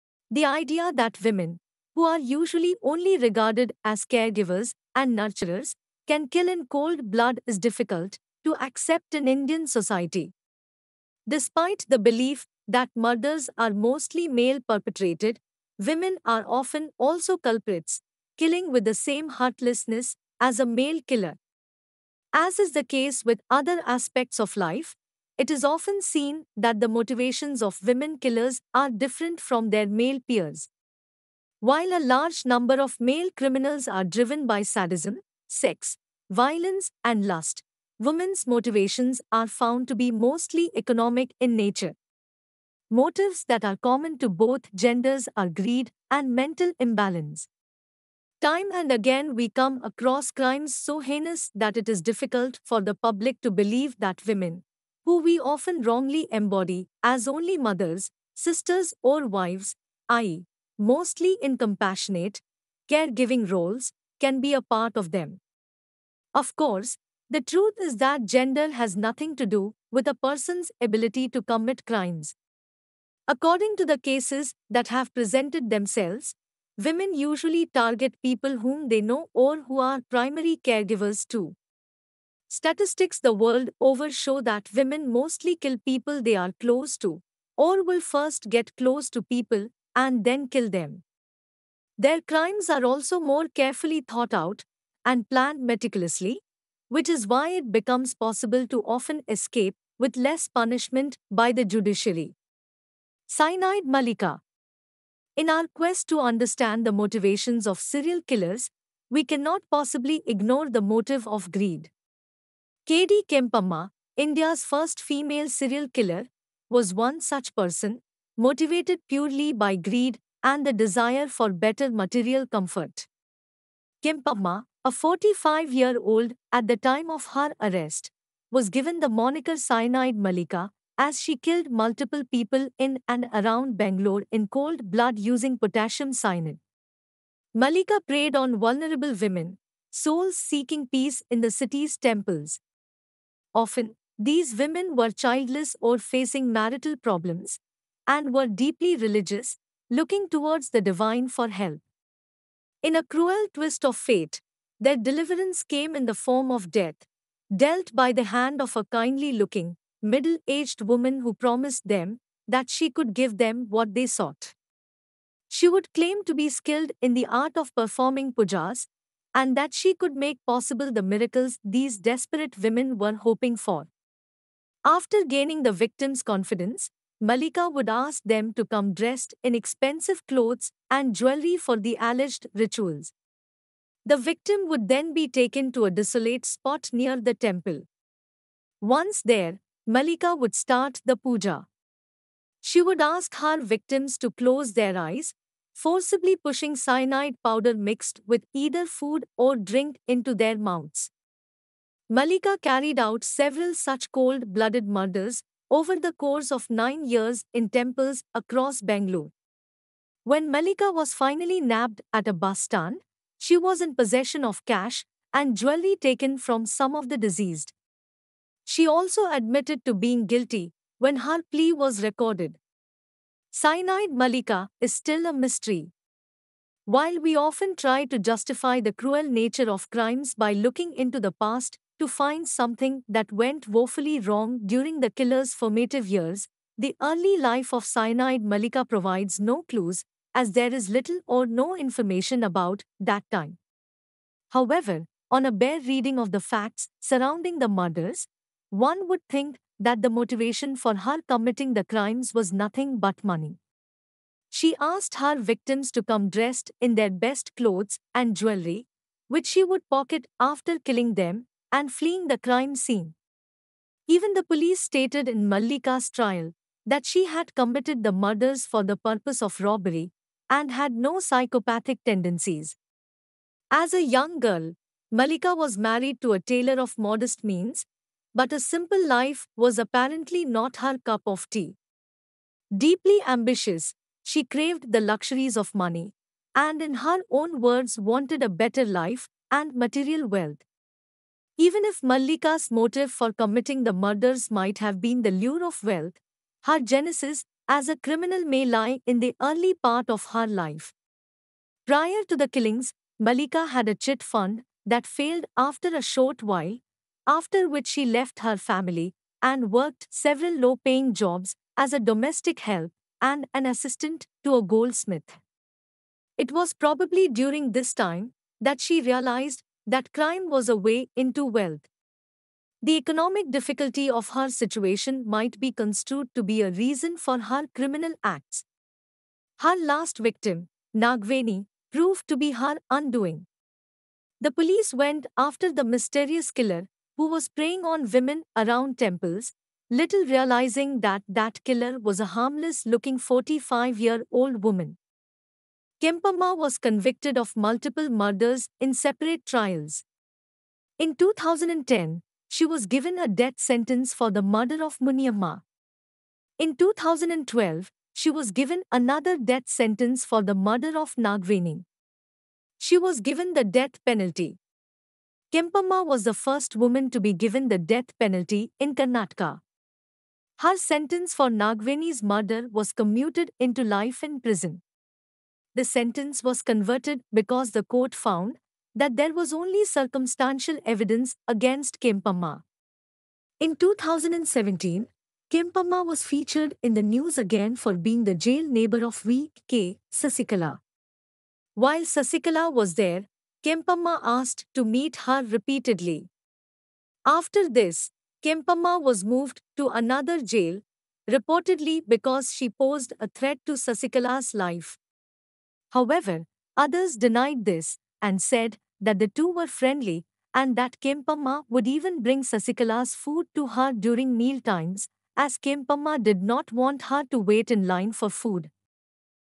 India's first woman serial killer
Kempamma was convicted of multiple murders in separate trials:

 In 2010, she was given a death sentence for the murder of Muniyamma:

 In 2012, she was given another death sentence for the murder of Nagaveni:

 She was given the death penalty:

 Kempamma was the first woman to be given the death penalty in Karnataka:

 Her sentence for Nagaveni's murder was commuted into life in prison:

 The sentence was converted because the court found that there was only circumstantial evidence against Kempamma:

In 2017, Kempamma was featured in the news again for being the jail neighbor of V. K. Sasikala:

While Sasikala was there, Kempamma asked to meet her repeatedly:

After this, Kempamma was moved to another jail, reportedly because she posed a threat to Sasikala's life:

However, others denied this and said that the two were friendly and that Kempamma would even bring Sasikala's food to her during meal times, as Kempamma did not want her to wait in line for food: